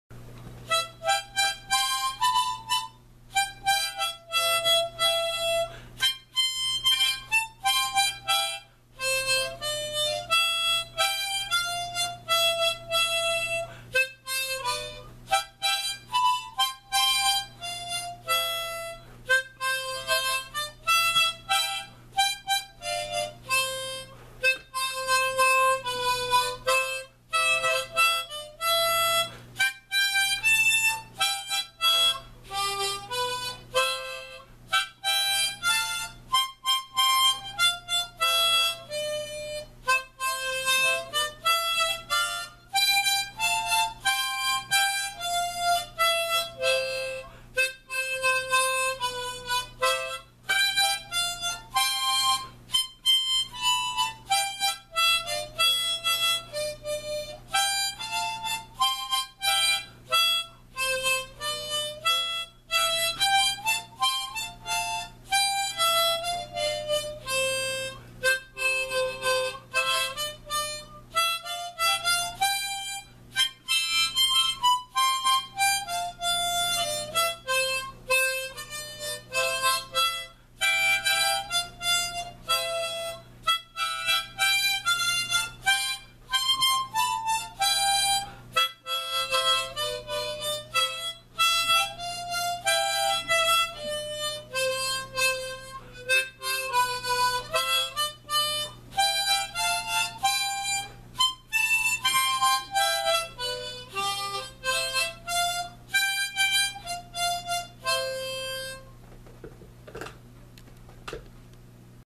Rebel Playing the Mississippi Brand Harmonica
Arnold Mackey of Kalamazoo,Michigan is playing a Mississippi Brand Harmonica.